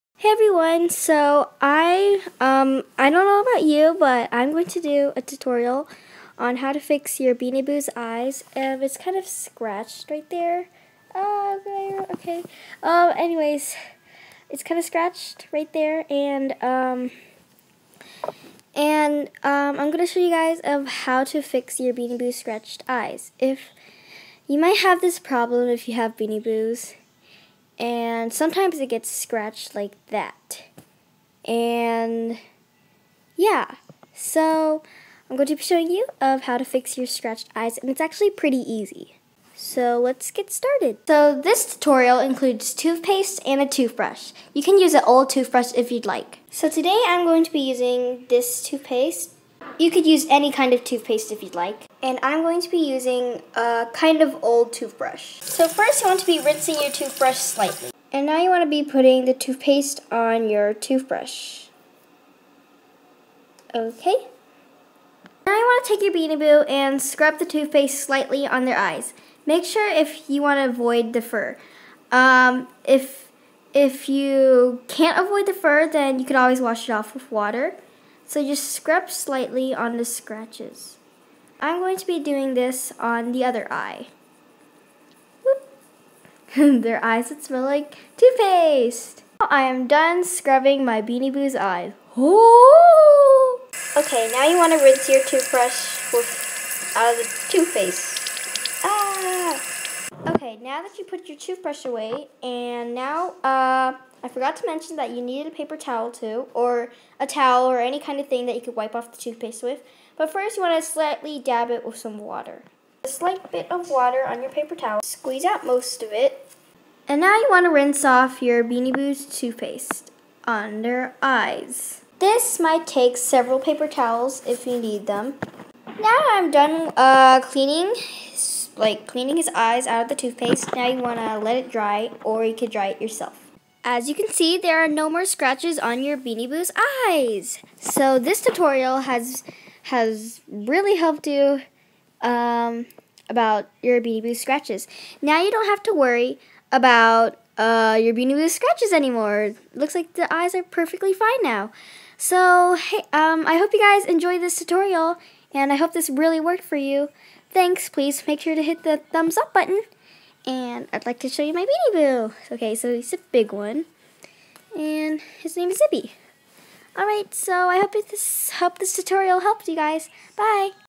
How to Fix your Beanie Boo's Scratched Eyes
Hey people! Today I am giving you this Tutorial of How to Fix your Beanie Boo's Scratched eyes! I hope it will work for you. Bye! :D
P.S. Sorry for being really hesitative in this video... I did not have a script written for what to say and I made videos poorly back then haha 8/1/2018

Materials I am using :
- aquafresh toothpaste
- old toothbrush
- paper towel